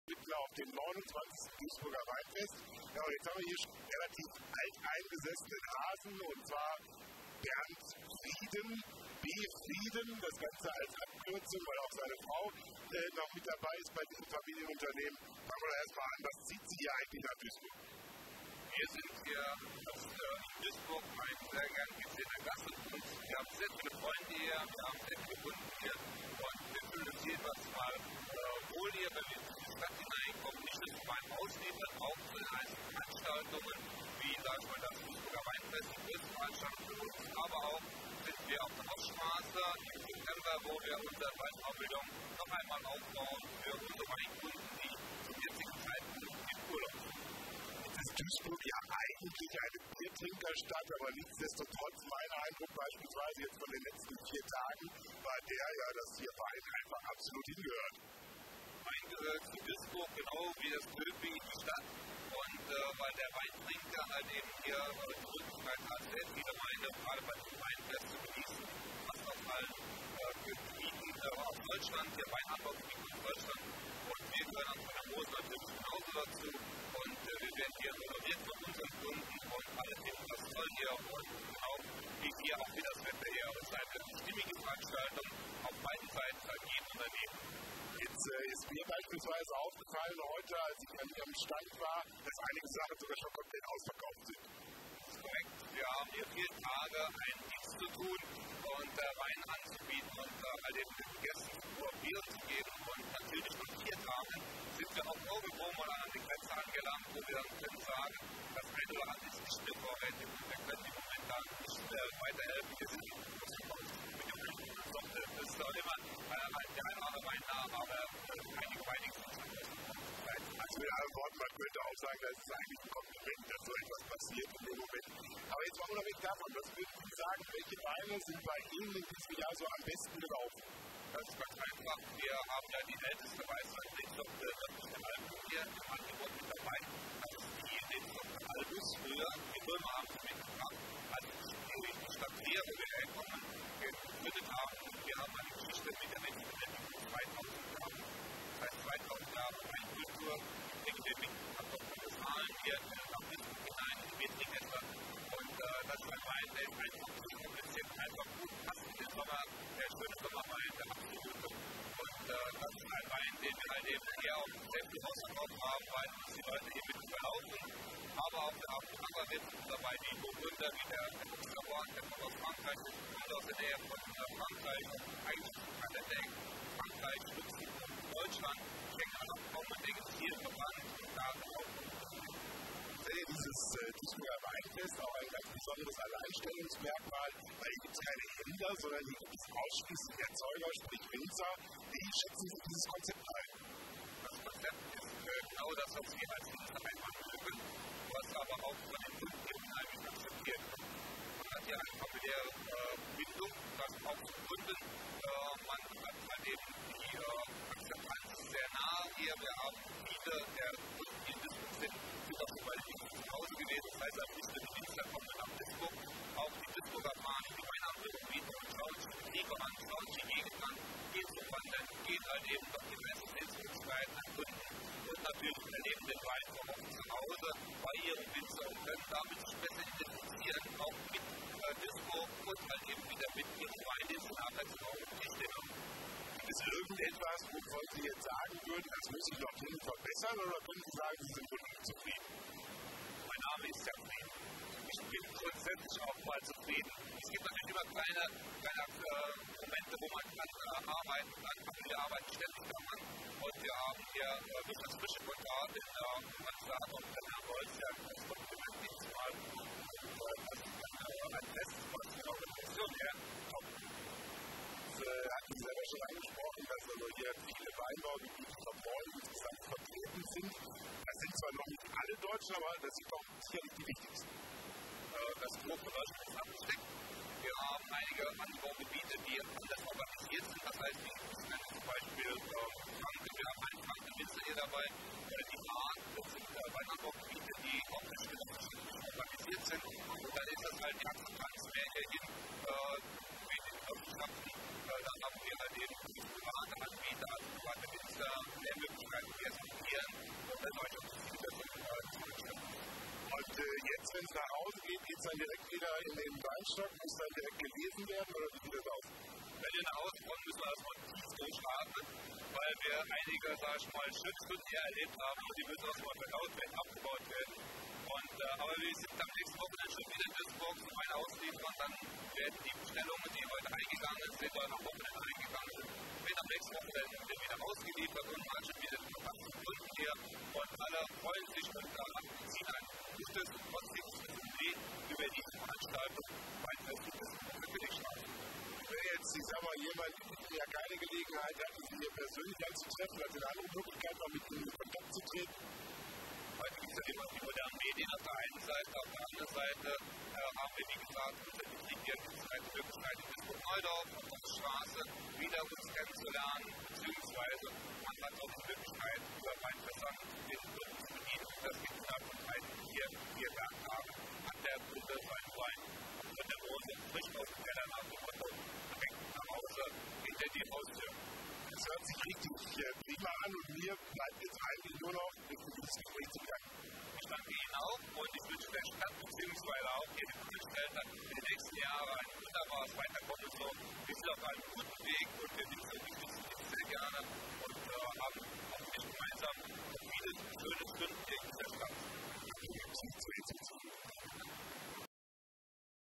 Mosel Winzer Bernd Frieden zum 29. Duisburger Weinfest
Der Winzer Bernd Frieden und seine Frau Birgit Frieden, Weinbautechnikerin und -laborantin kommen aus dem Weinort Nittel an der deutsch-luxemburgischen Grenze und sind bereits seit vielen Jahren auf dem Weinfest in Duisburg mit ihren zahlreichen Weinen vor Ort. Für sie hat dieses Weinfest inmitten der Biertrinker-Stadt Tradtion und gehört als wichtiger Bestandteil zu dieser Stadt. Sie kommen gerne her und und haben in den vergangenen Jahren viele Freundschaften nach Duisburg geknüpft. Natürlich laden sie ihre Gäste auch gerne in ihr Gästehaus auf dem Weingut an der Mosel ein, um sich vor Ort im Rahmen eines kleinen Urlaubs einmal einen persönlichen Eindruck zu verschaffen. Rundum zufrieden ist Bernd Frieden mit dem 29. Duisburger Weinfest und bedankt sich ausdrücklich bei dem im FrischeKontor zuständigen Mitarbeiter Günter Bolz für die einmal mehr hervorragend durchgeführte Organisation der Veranstaltung. Wir hatten die Gelegenheit mit dem Weinerzeuger von der Mosel ein ausführliches Gespräch zum Ende des Weinfestes am Sonntag, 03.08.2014 nach vier Tagen zu führen. Hören und sehen Sie mehr dazu in unserem angefügten Videobeitrag von der Königstrasse in Duisburg Stadtmitte.
Gespräch und Realisation: Frank-M. Fischer,